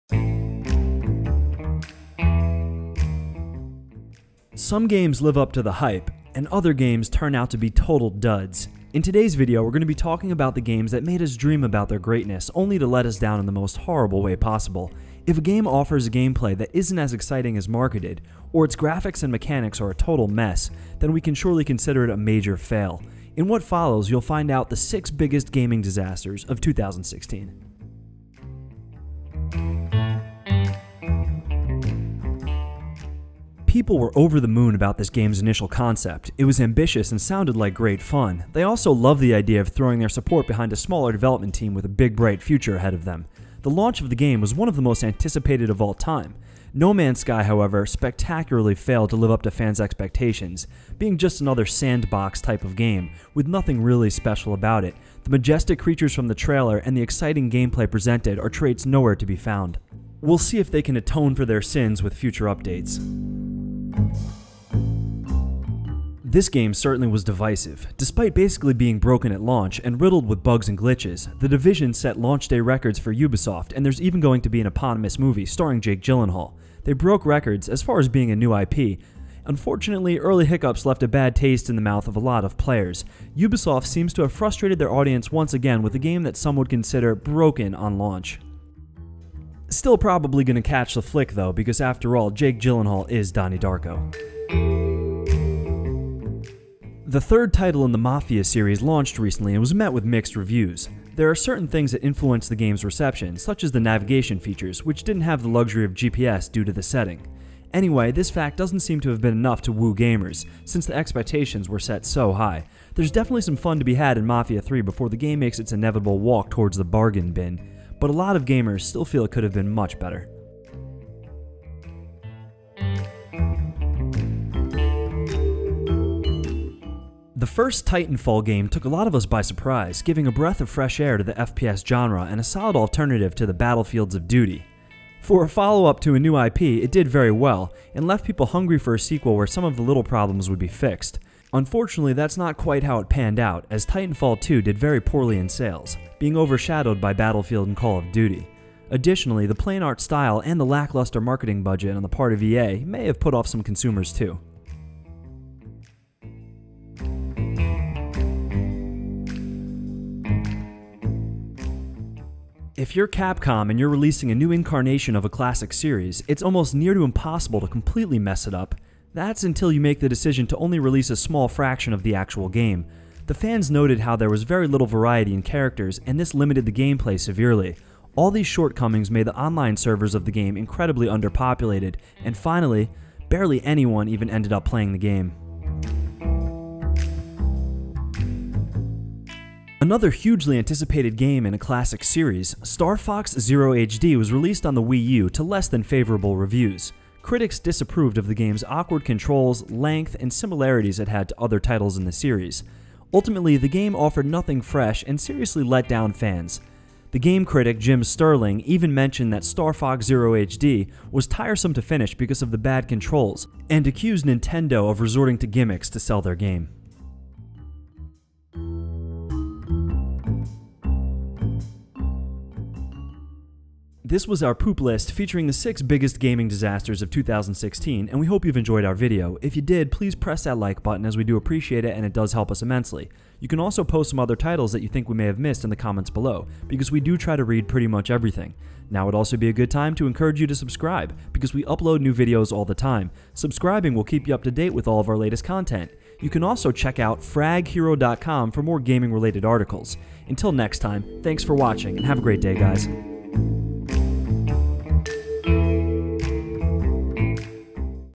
6 Disappointing Games Released in 2016 That Were Disasters
There are games that live up to the expectation and there are games that turn up to be total disasters. In today’s video we are going to talk about the games that made us dream about their greatness only to let us down in the most horrible way possible. If a game offers a gameplay that isn’t as exciting as marketed, or its graphics and mechanics are a total mess, then we can surely consider it a major fail. In what follows you’ll find out what are the 6 biggest gaming disasters of 2016.